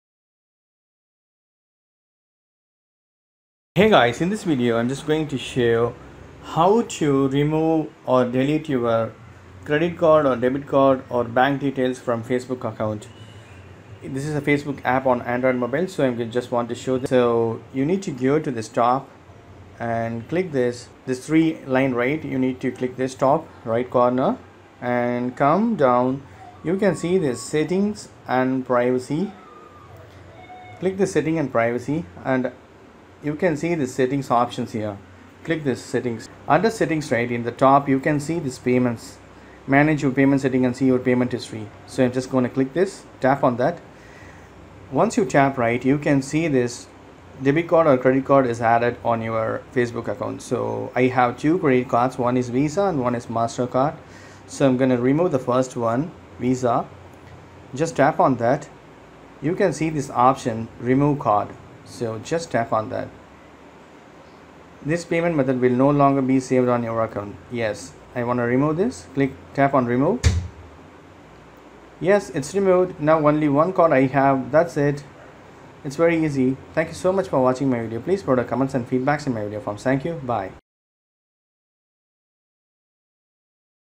How to DELETE or REMOVE your Credit Card or Debit Card details from Android Mobile on Facebook app?
Hey Guys, in this video i am showing How to DELETE or REMOVE your Credit Card or Debit Card details on Facebook app from android mobile or any android devices.